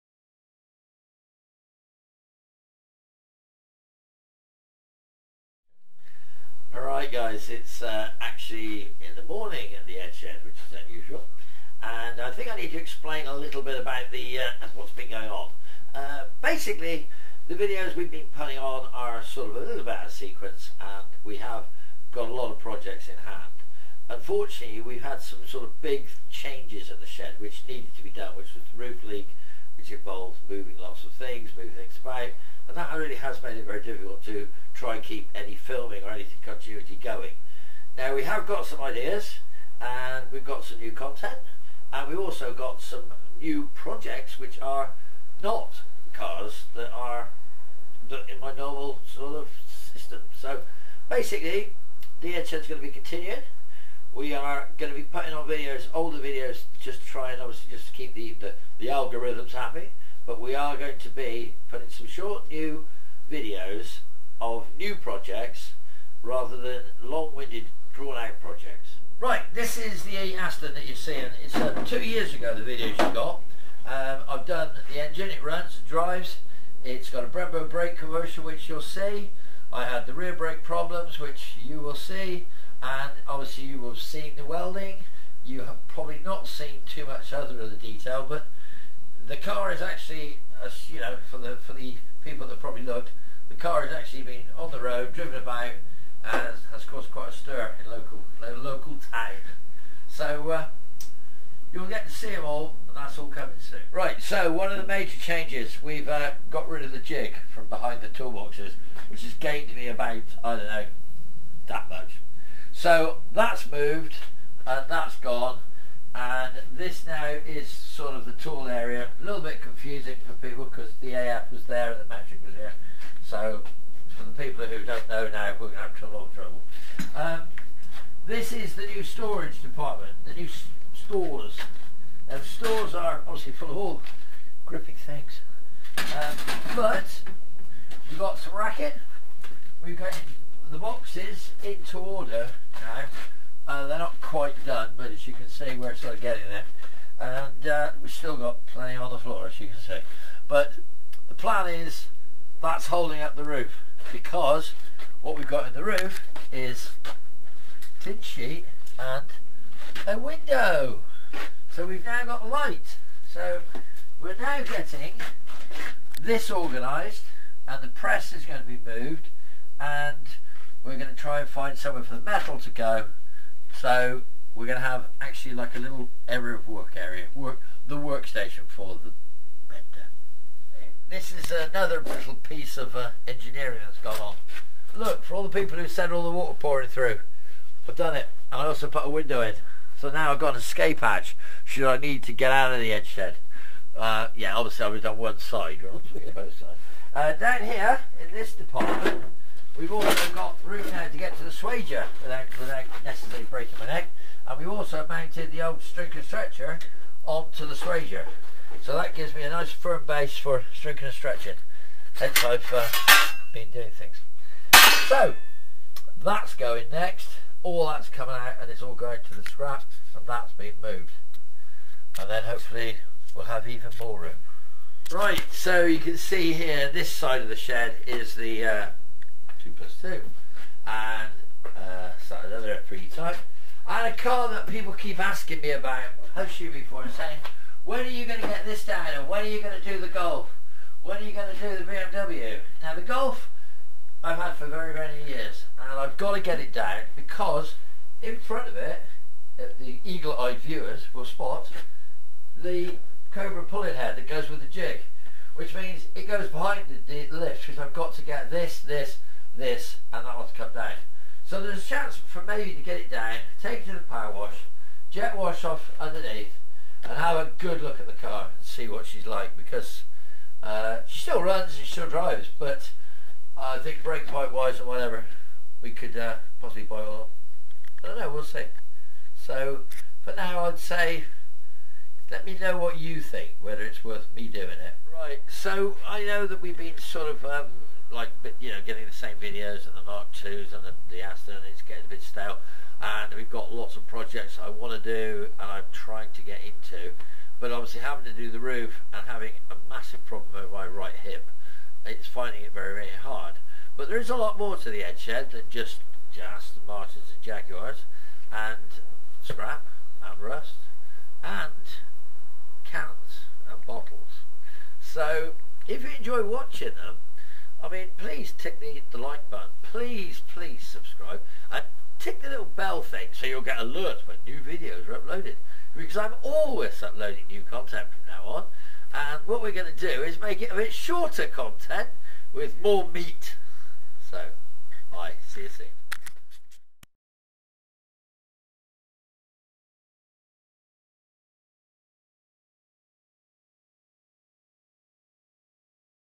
Aston Martin DBS V8, VW Mk2 Golf GTD, Roofing and a tidy up... An Update on Whats New at the EdShed
The EdShed has some infrastructure work done including some roofing and a good tidy up plus we are getting down to some jobs on the Aston Martin and some plans for the Golf GTD that has been flying in the shed for many years...